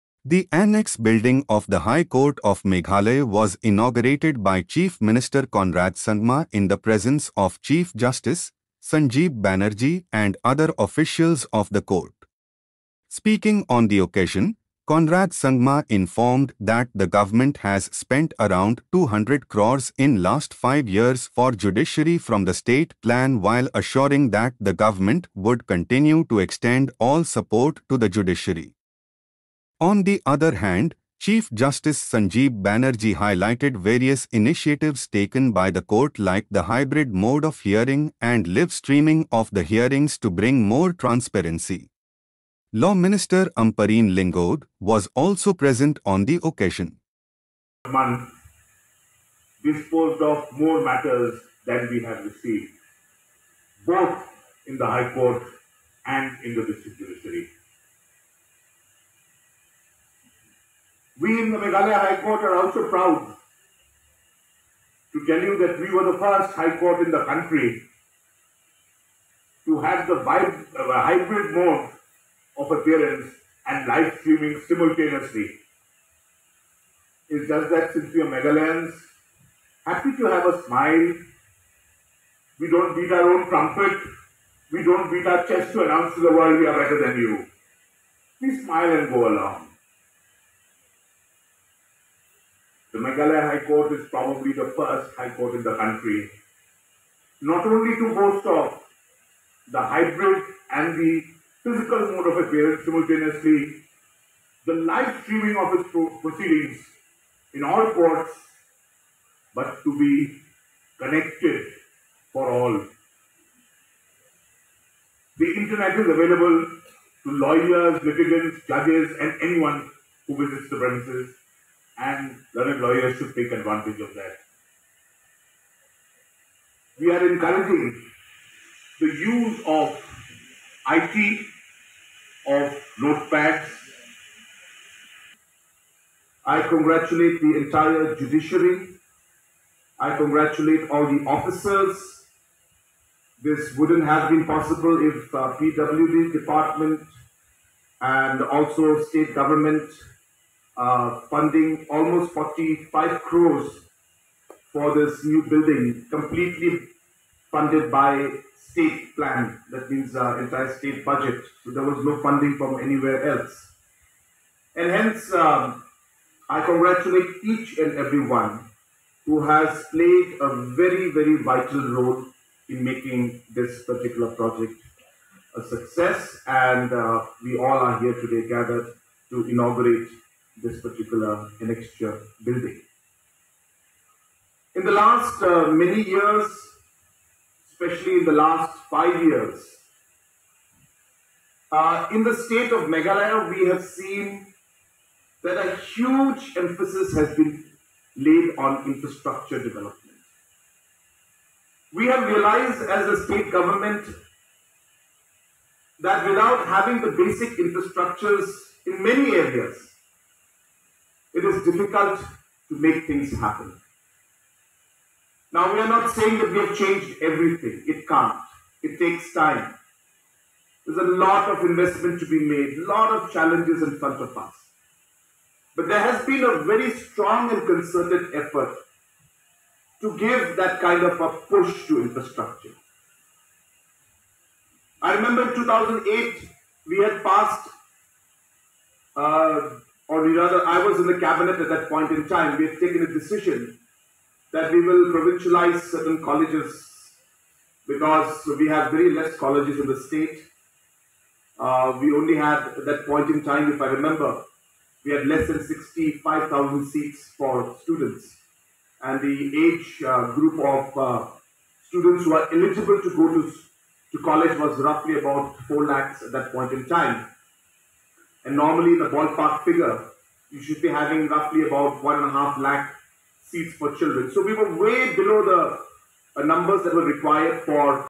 Meghalaya High Court annexe building inaugurated by Chief Minister Conrad Sangma
The annexe building of the High Court of Meghalaya was inaugurated by Chief Minister Conrad Sangma in  the presence of Chief Justice, Sanjib Banerjee and other officials of the Court. Speaking on the occasion,  Conrad Sangma informed that the Government has spent around  200 crores in last 5 years for judiciary from the state plan while assuring that the Government would continue to extend all support to the judiciary.  On the  other hand, Chief Justice Sanjib Banerjee highlighted various initiatives taken by the Court  like the Hybrid mode of hearing and live streaming of the hearings to bring more transparency.  Law Minister Ampareen Lyngodh was also  present on the occasion